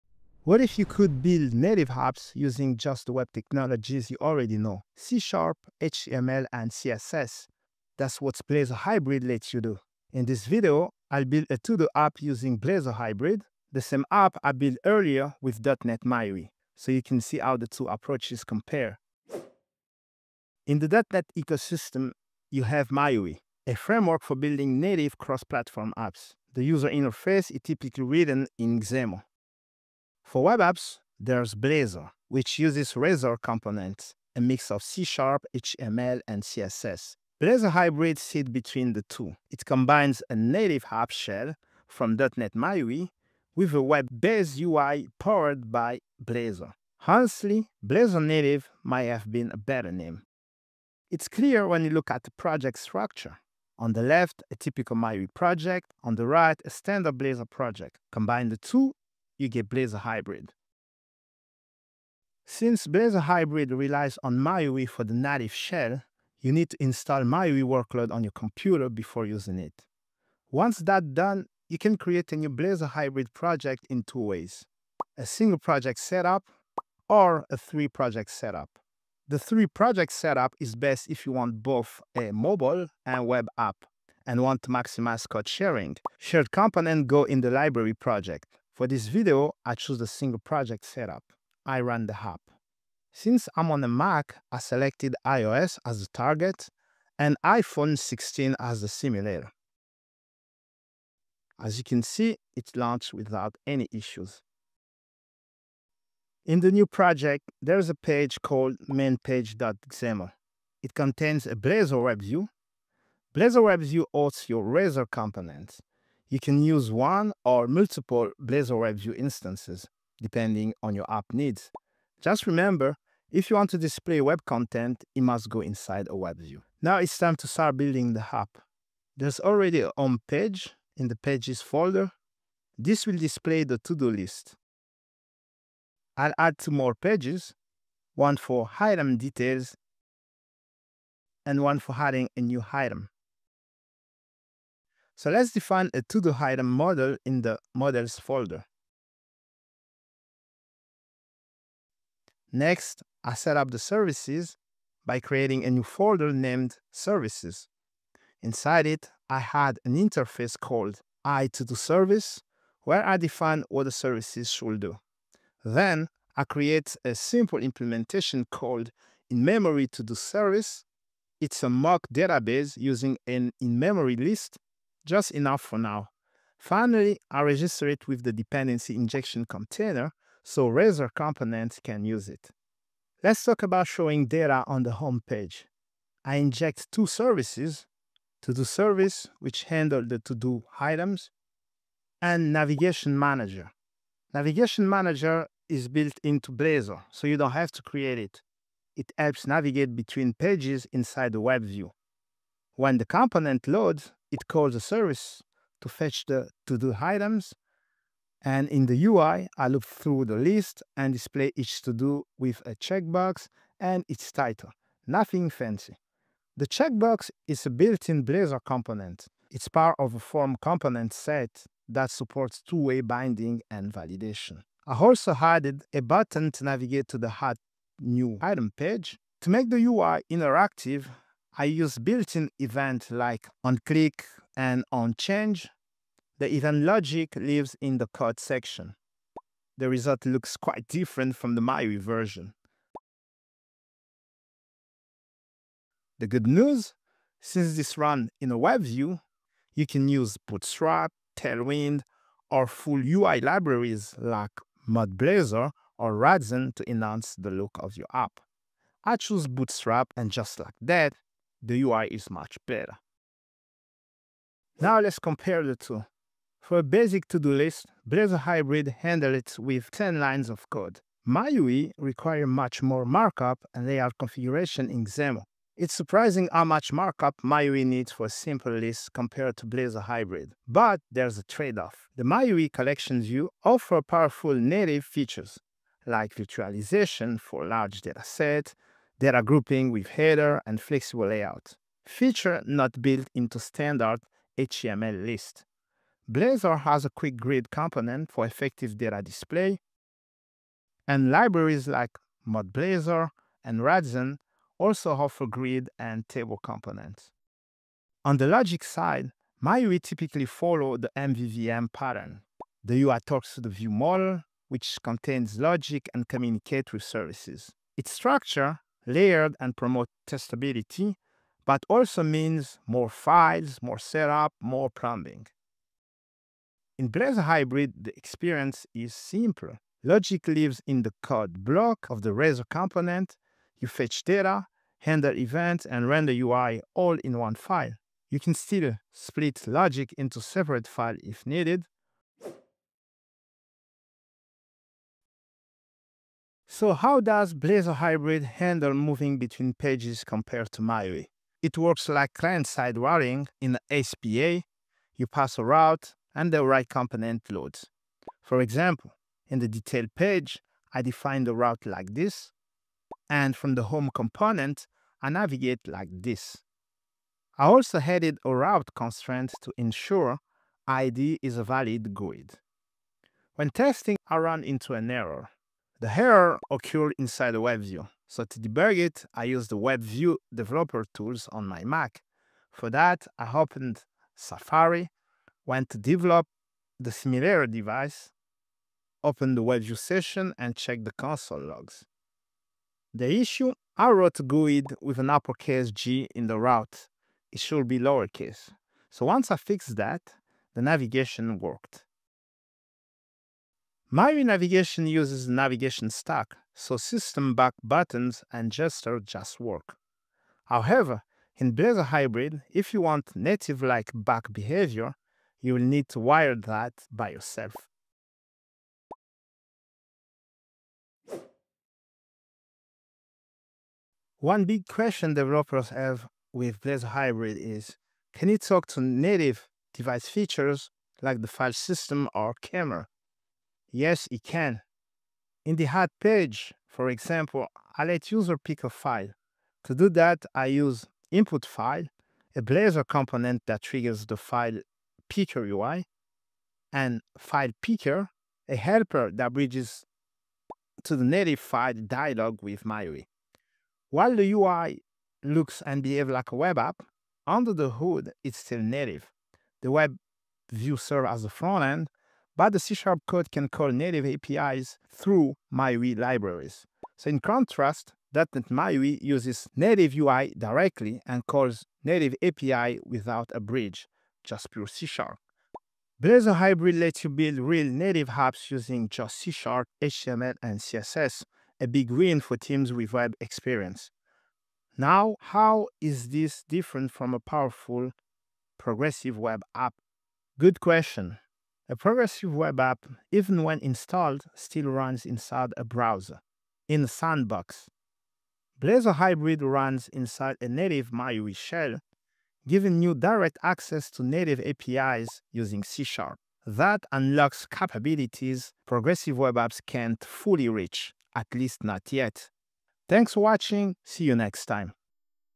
I Used Blazor Hybrid Instead of MAUI — Here’s What Changed
Learn how I built the same Todo app using Blazor Hybrid and .NET MAUI